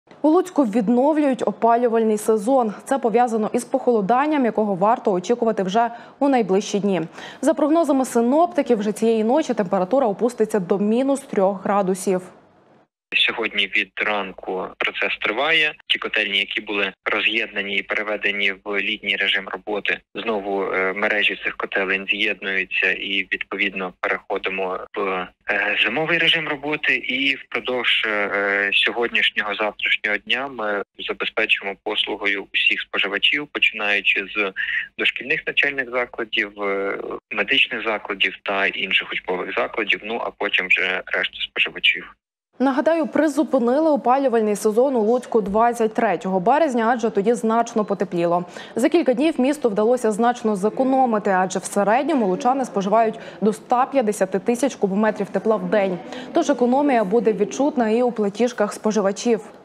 Через похолодання у Луцьку відновлюють опалювальний сезон
У Луцьку відновлюють опалювальний сезон. Це пов'язано із похолодонням, якого варто очікувати вже у найближчі дні. За прогнозами синоптиків, вже цієї ночі температура опуститься до мінус трьох градусів.
12 Канал у телеграмі, усі важливі новини Волині, України і світу.